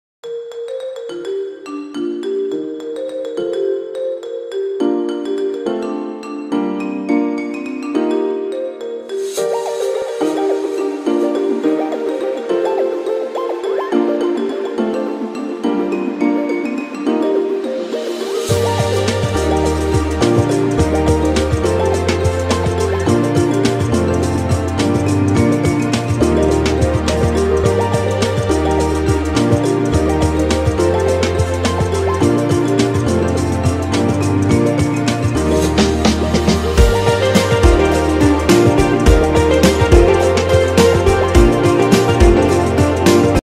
CHIMIA - Green Day
ChiMia is participating in the Green Freaks Hunt and Sidewalk Sale. Which is a grid-wide hunt & sidewalk sale that celebrates all things GREEN! The sale runs March 13th-27th.

Items in the photo in the Green Freaks Hunt and Sidewalk Sale are:

    Midcentury Console (Green)
    Tarovine Potted Plant
    Mary Jane Hall Tree [Mint]
    St. Patrick's Balloons
    House Plant

Midcentury Console
The Midcentury Console Pack comprises 5 colour variants packaged separately.
Each console is 2 land impact each.

Camping Tent
These creations won’t be available at the ChiMia main store or marketplace store.

neutral colored fabric and wood tent structure with wooden floor base. The spacious interior leaves plenty of room to add your own furniture and decor and still have room for mutliple avatars.

Original mesh by our own Jack (Markus Slingshot).

162 land impact.

Copy and mod, no transfer. This means you can rez as many as you want within land impact limits plus edit the object’s size, name, contents list, etc.